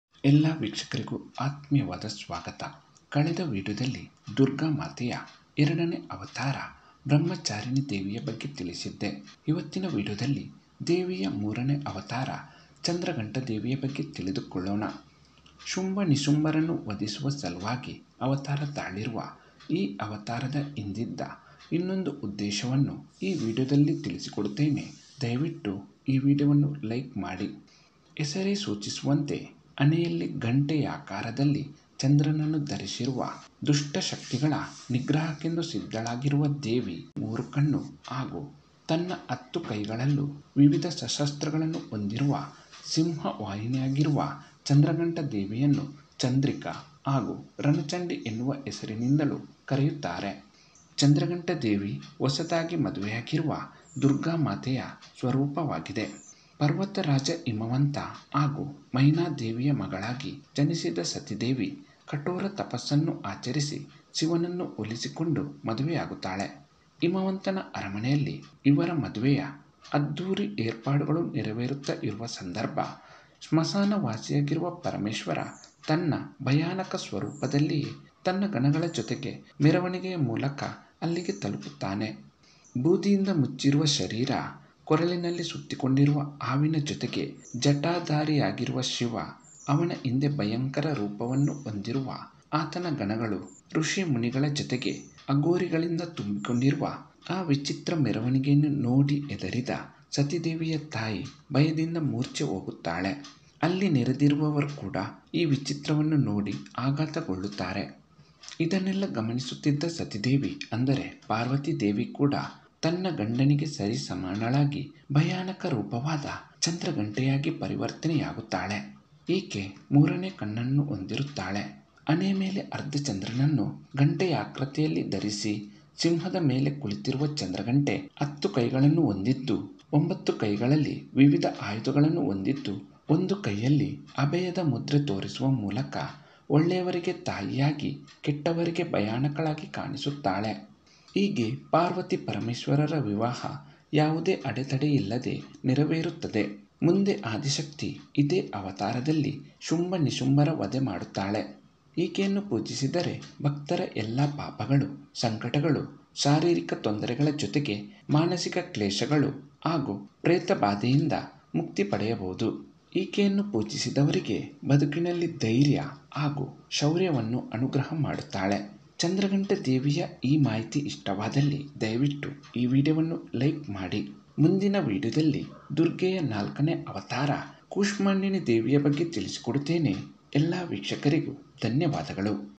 ದುರ್ಗಾ ದೇವಿಯ 3 ನೇ ಅವತಾರ ಚಂದ್ರಘಂಟ ದೇವಿಯ ಚರಿತ್ರೆ | Chandraghanta Devi Charitre
Navaratri story in kannada

ದುರ್ಗಾ ದೇವಿಯ ಈ ಉಗ್ರರೂಪದ ಅವತಾರದ ಹಿಂದೆ ಇದೆ ಎರಡು ಮುಖ್ಯ ಕಾರಣಗಳು
Durga Devi stories in kannada

ಈ ವಿಡಿಯೋದಲ್ಲಿ ಉಪಯೋಗಿಸಲಾದ Images(ಭಾವಚಿತ್ರಗಳು) ಕೇವಲ ಮಾಹಿತಿ ನೀಡುವ ಸಲುವಾಗಿ ಹಾಗೂ ವಿಡಿಯೋ ದ ಪಾತ್ರಗಳ ಬಗ್ಗೆ ಮನದಟ್ಟು ಮಾಡುವ ಒಳ್ಳೆಯ ಉದ್ದೇಶಕ್ಕೆ ಈ ಭಾವಚಿತ್ರ ಗಳನ್ನು ಉಪಯೋಗಿಸಲಾಗಿದೆ..
Photos used for only educational purpose.